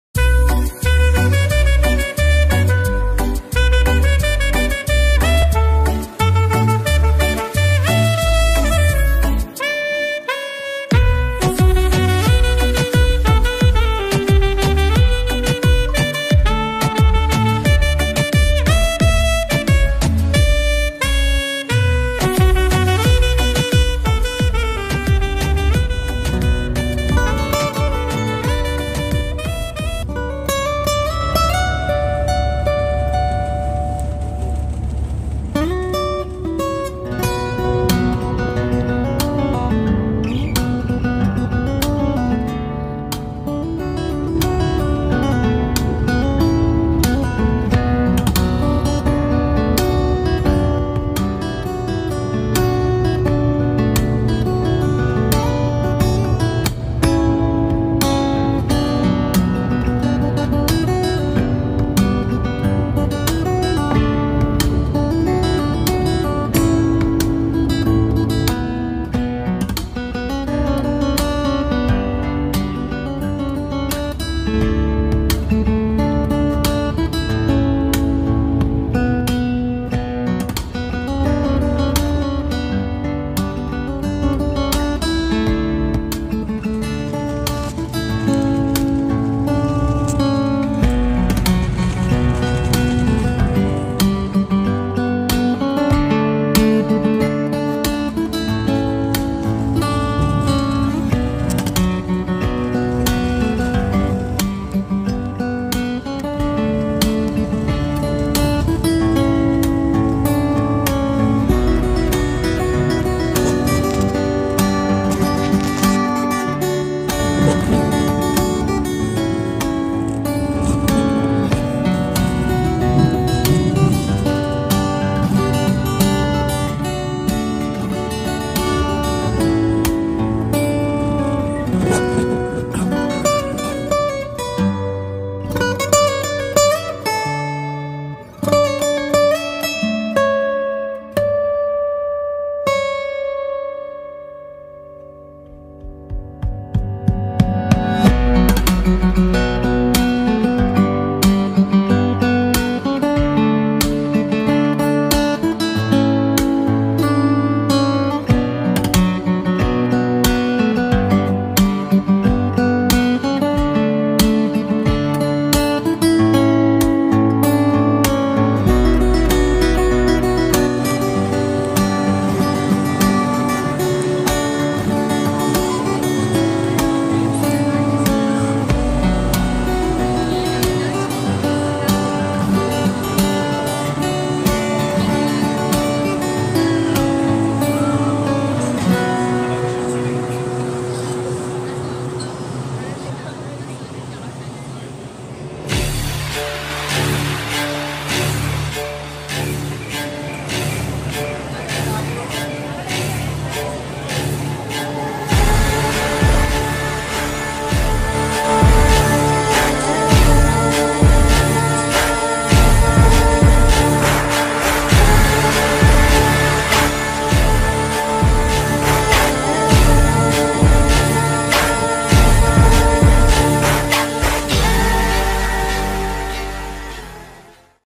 cheapest Road trip from holiday Bangkok Thailand kualalampurmalaysia mercedes tour hotels funny
Hatyai to KL by bus. City tour of malaysia and sky view of KLCC from KL TOWER#cheapest